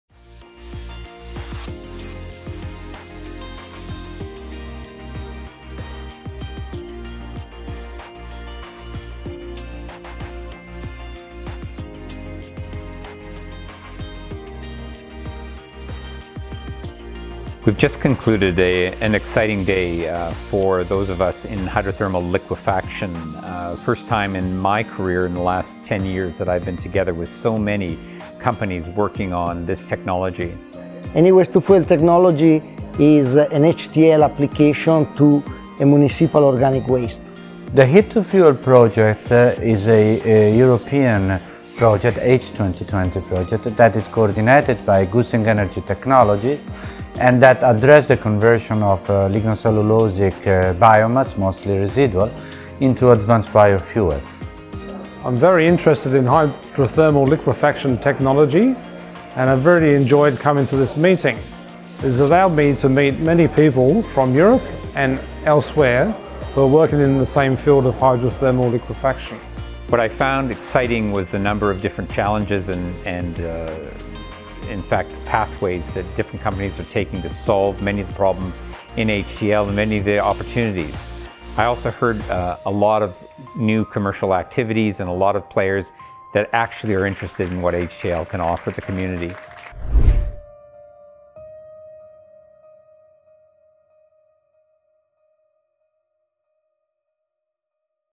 Workshop ‘Potential of Hydrothermal Liquefaction (HTL) routes for biofuel production’ – Nov 19, 2019
This video is about the Expert workshop on ‘Potential of Hydrothermal Liquefaction (HTL) routes for biofuel production’, held in Brussels (B). It was co-organized by five European H2020 projects and a Norwegian FME research center. The workshop attracted great interest: almost 100 participants attended from industry, SMEs, EC, research organizations, universities, and other entities from all across the world. These Projects have received funding from the European Union’s Horizon 2020 research and innovation programme under grant agreements No 764734 (HyFlexFuel), No 818120 (WASTE2ROAD), No 818413 (NextGenRoadFuels), No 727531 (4REFINERY), No 764675 (Heat to Fuel).